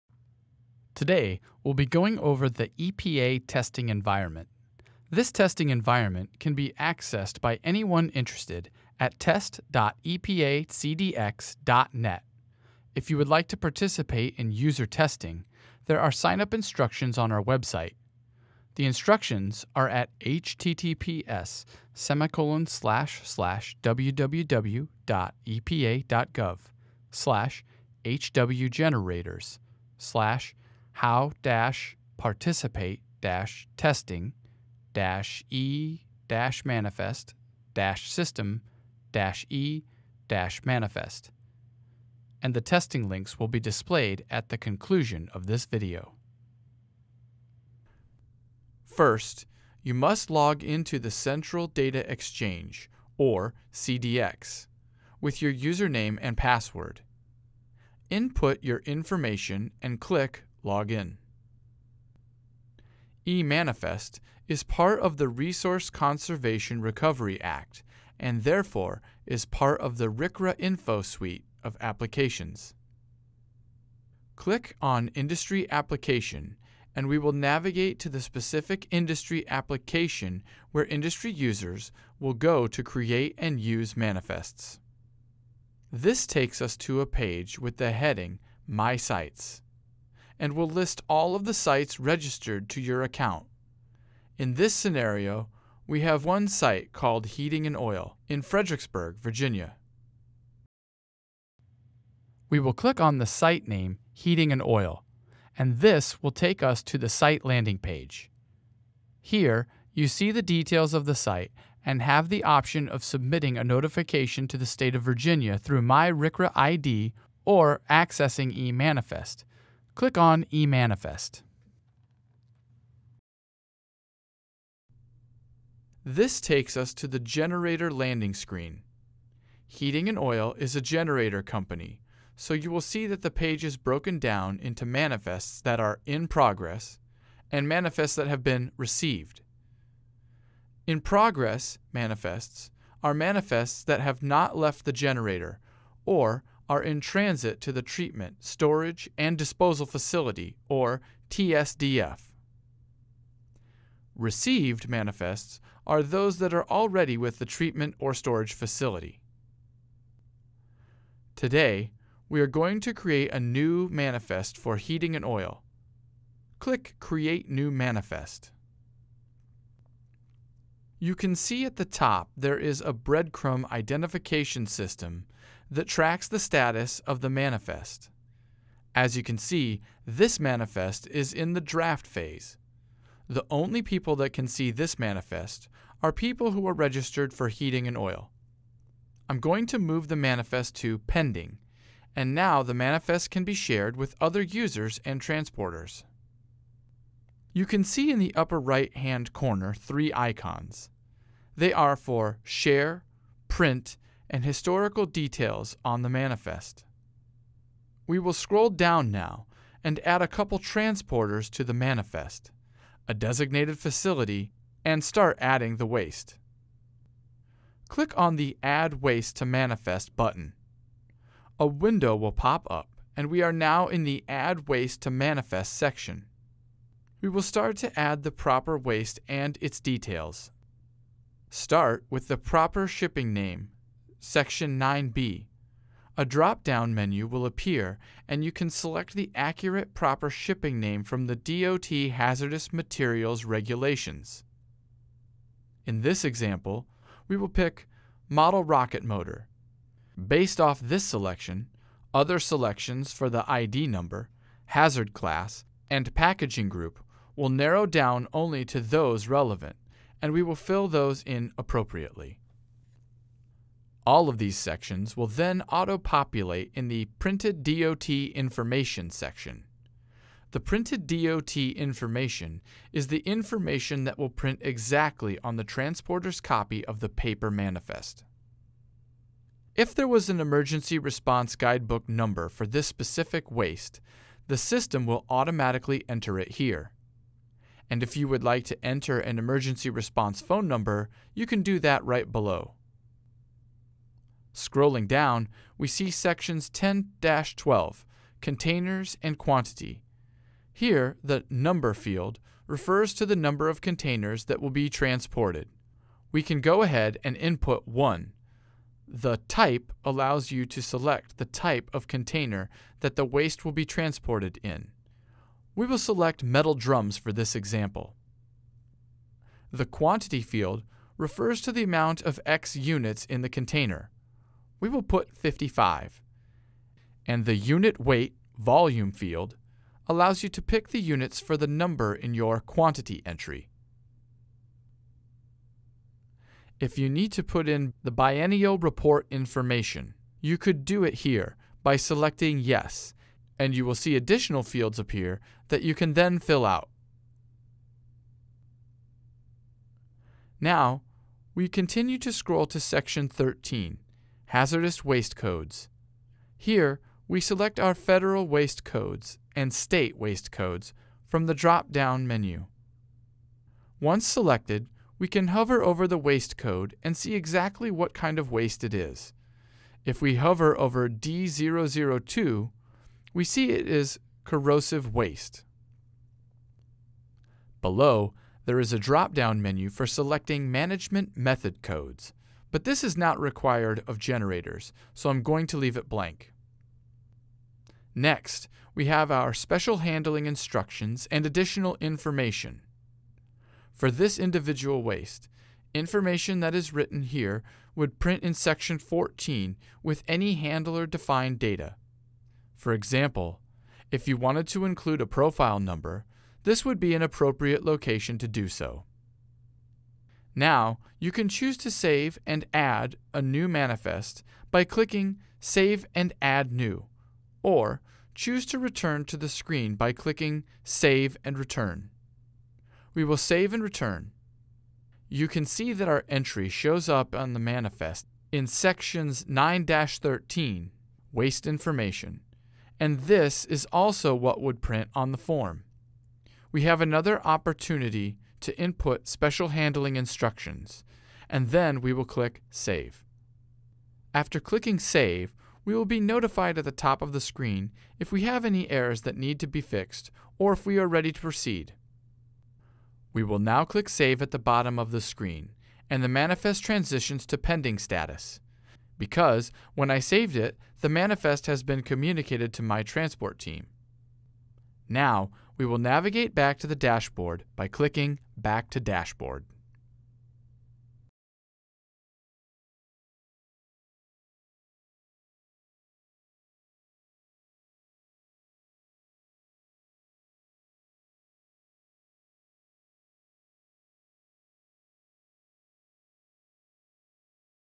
The Hazardous Waste Electronic Manifest (e-Manifest) System Walkthrough
EPA held this webinar in March 2017 for hazardous waste generators that wish to participate in testing the e-manifest system. System developers held a demo, which you can watch in the video above.